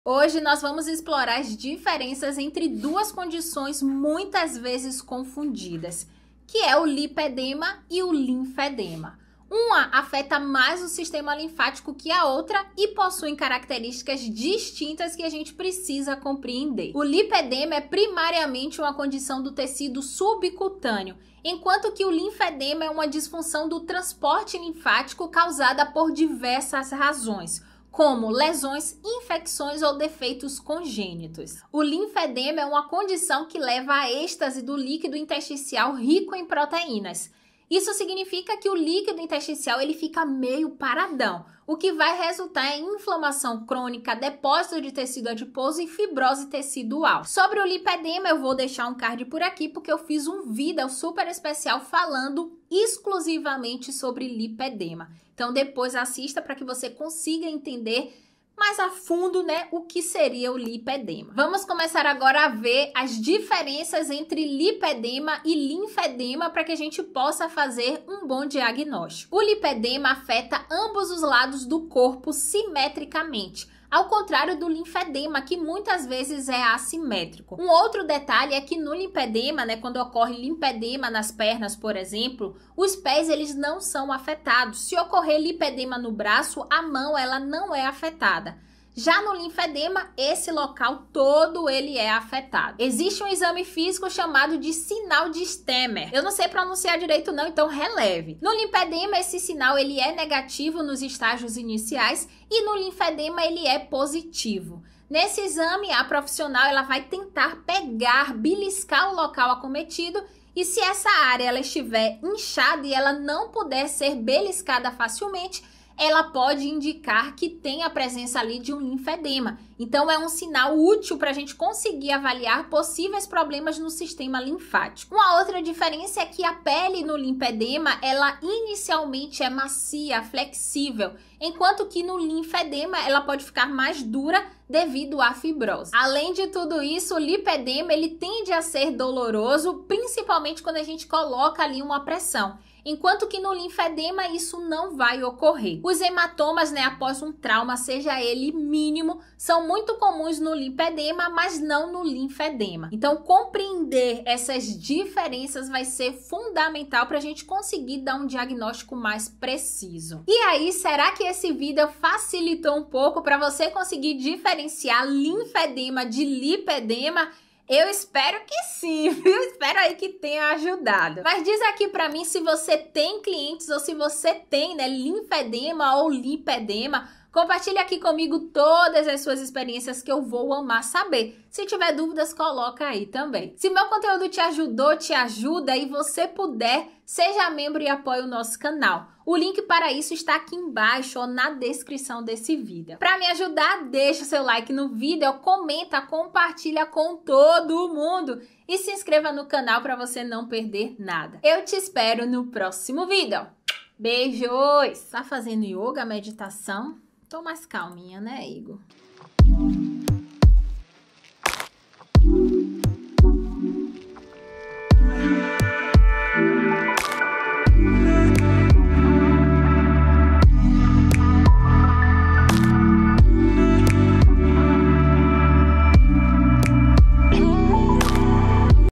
Lipedema Vs Linfedema: Qual a diferença?
🤔✨ Descubra as Diferenças Cruciais entre Lipedema e Linfedema! 👀🔍

Hoje vamos explorar essas duas condições frequentemente confundidas, mas com características distintas que precisamos entender. O Lipedema é uma condição do tecido adiposo, enquanto o Linfedema é uma disfunção linfática.

🔬 No diagnóstico, destaca-se que o Lipedema afeta simetricamente ambos os lados do corpo, enquanto o Linfedema pode ser assimétrico. O sinal de Stemmer é um exame útil: negativo no Lipedema e positivo no Linfedema, indicando problemas no sistema linfático.

👐 Outras diferenças incluem a textura da pele (macia no Lipedema, mais dura no Linfedema), sensibilidade à pressão (dor no Lipedema, não no Linfedema), e a ocorrência de hematomas após trauma mínimo, comum no Lipedema, mas não no Linfedema.

👩‍⚕️ Compreender essas nuances é crucial para um diagnóstico preciso. Assista ao vídeo para mais informações sobre essas condições e como identificá-las corretamente! 🎥🩹

ツ 𝗔𝗽𝗼𝗶𝗲 𝗻𝗼𝘀𝘀𝗼 𝗰𝗮𝗻𝗮𝗹:
Seja membro deste canal e ganhe benefícios!

Ps.: Não usamos o grupo e canal para fazer vendas!

Capítulos:
00:00 - Introdução
00:18 - Qual local o lipedema e linfedema afeta?
00:32 - O que é linfedema?
01:04 - Quais diferenças entre linfedema e lipedema?